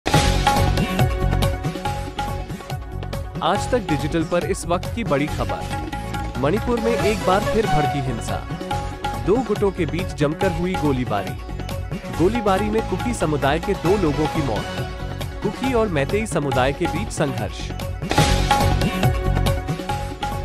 Breaking News: Manipur में दो गुटों के बीच जमकर हुई गोलीबारी, कुकी समुदाय के 2 लोगों की मौत
मणिपुर में जारी हिंसा थमने का नाम नहीं ले रही. यहां कुकी और मैतेई समुदाय के बीच संघर्ष का दौर जारी है. जानकारी के मुताबिक 2 सशस्त्र उपद्रवी समूहों के बीच एक बार फिर गोलीबारी हुई है. बताया जा रहा है कि उपद्रवियों ने तीन जिलों कांगपोकपी, उखरूल और इंफाल पूर्व के ट्राइजंक्शन जिले में एक दूसरे पर फायरिंग की.  गोलीबारी में कुकी समुदाय के 2 लोगों की मौत हो गई.